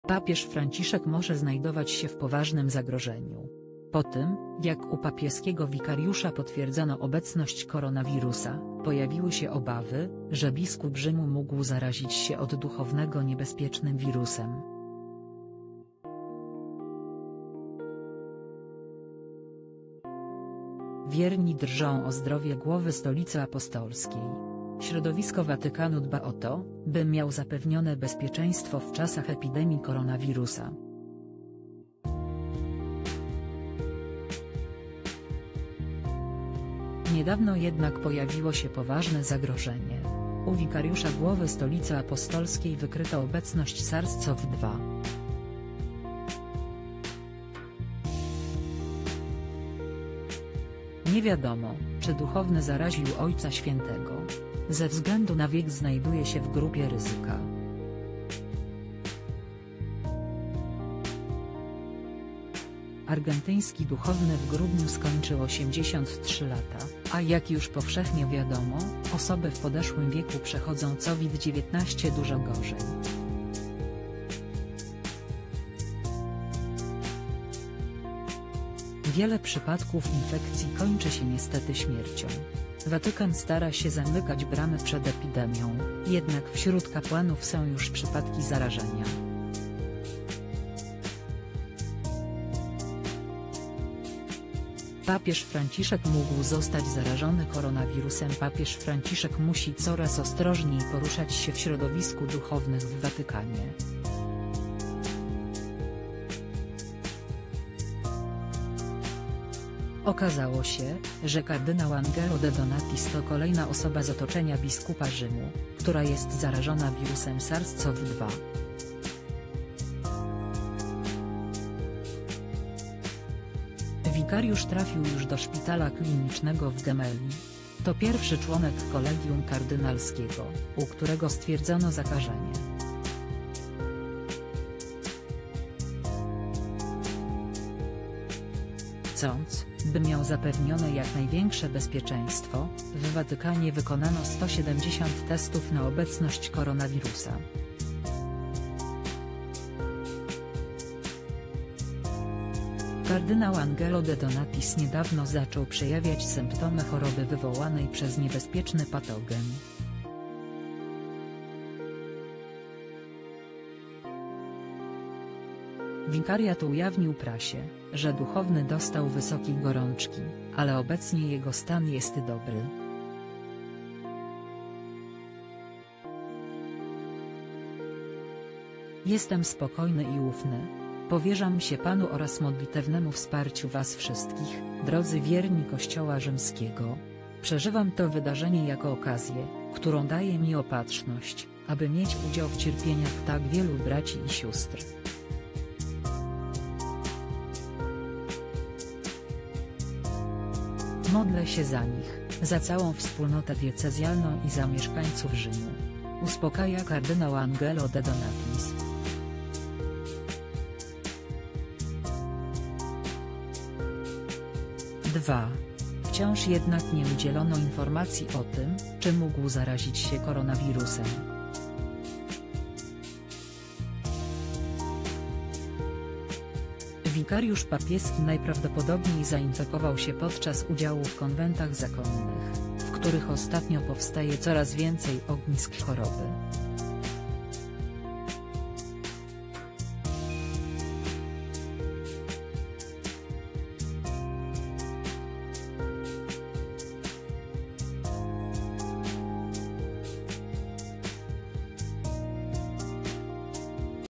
Watykański wikariusz zaraził papieża Franciszka? Jest chory, władze Stolicy Apostolskiej pełne obaw
Papież Franciszek może znajdować się w poważnym zagrożeniu. Po tym, jak u papieskiego wikariusza potwierdzono obecność koronawirusa, pojawiły się obawy, że biskup Rzymu mógł zarazić się od duchownego niebezpiecznym wirusem. Wierni drżą o zdrowie głowy Stolicy Apostolskiej.   Środowisko Watykanu dba o to, by  miał zapewnione bezpieczeństwo w czasach epidemii koronawirusa. Niedawno jednak pojawiło się poważne zagrożenie. U wikariusza głowy Stolicy Apostolskiej wykryto obecność SARS-CoV-2. Nie wiadomo, czy duchowny zaraził ojca świętego.   Ze względu na wiek  znajduje się w grupie ryzyka. Argentyński duchowny w grudniu skończył 83 lata, a jak już powszechnie wiadomo, osoby w podeszłym wieku przechodzą COVID-19 dużo gorzej. Wiele przypadków infekcji kończy się niestety śmiercią. Watykan stara się zamykać bramy przed epidemią, jednak wśród kapłanów są już przypadki zarażenia.  Papież Franciszek mógł zostać zarażony koronawirusem   Papież Franciszek musi coraz ostrożniej poruszać się w środowisku duchownych w Watykanie. Okazało się, że kardynał Angelo De Donatis to kolejna osoba z otoczenia biskupa Rzymu, która jest zarażona wirusem SARS-CoV-2. Wikariusz trafił już do szpitala klinicznego w Gemelli. To pierwszy członek kolegium kardynalskiego, u którego stwierdzono zakażenie.   Chcąc, by  miał zapewnione jak największe bezpieczeństwo, w Watykanie wykonano 170 testów na obecność koronawirusa. Kardynał Angelo De Donatis niedawno zaczął przejawiać symptomy choroby wywołanej przez niebezpieczny patogen. Wikariat ujawnił prasie, że duchowny dostał wysokiej gorączki, ale obecnie jego stan jest dobry.      - Jestem spokojny i ufny. Powierzam się Panu oraz modlitewnemu wsparciu was wszystkich, drodzy wierni Kościoła Rzymskiego! Przeżywam to wydarzenie jako okazję, którą daje mi Opatrzność, aby mieć udział w cierpieniach tak wielu braci i sióstr. Modlę się za nich, za całą wspólnotę diecezjalną i za mieszkańców Rzymu! - uspokaja kardynał Angelo De Donatis.       2.    Wciąż jednak nie udzielono informacji o tym, czy  mógł zarazić się koronawirusem. Wikariusz papieski najprawdopodobniej zainfekował się podczas udziału w konwentach zakonnych, w których ostatnio powstaje coraz więcej ognisk choroby.